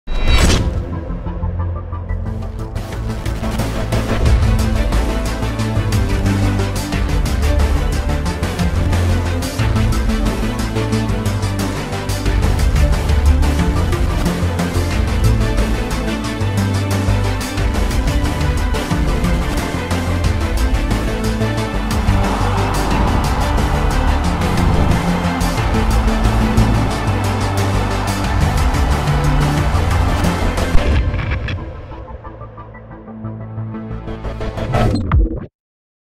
ELREENCUENTRO - Uruguay-Costa Rica - 13/11 - 21 h - Estadio Centenario. AUF
La rambla de Montevideo, el Obelisco, el Palacio Salvo, el Teatro Solís, la Puerta de la Ciudadela, un ómnibus y el Estadio Centenario fueron los lugares elegidos para proyectar imágenes de la selección en este spot de la AUF para promocionar el amistoso entre Uruguay y Costa Rica.